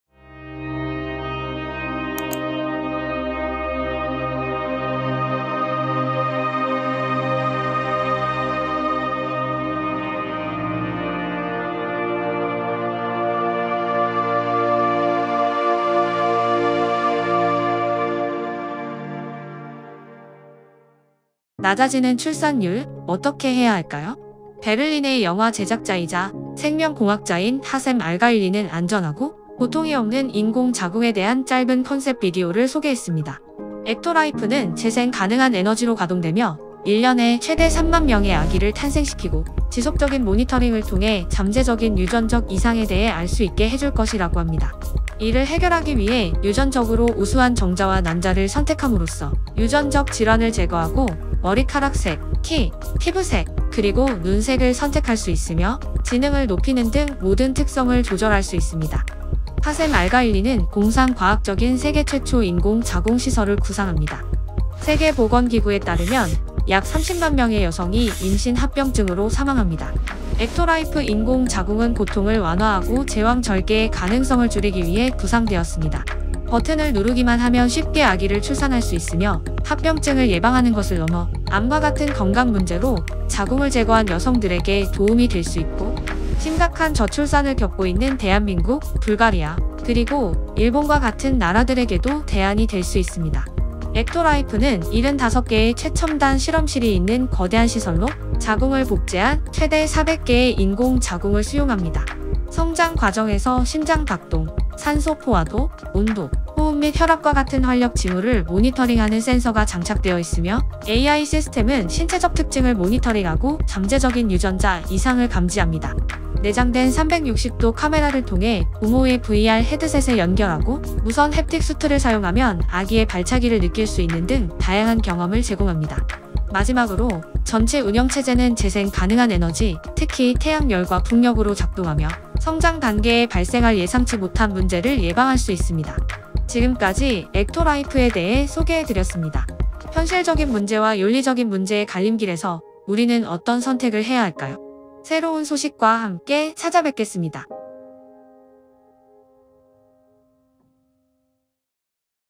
인간은 신이 될 수 있을까. 인공 자궁 EctoLife
AI 및 새로운 기술에 대한 뉴스를 전해드리고자 합니다.
오늘 소개해 드릴 신기술은 EctoLife입니다.
오역 및 의역이 있을 수 있습니다.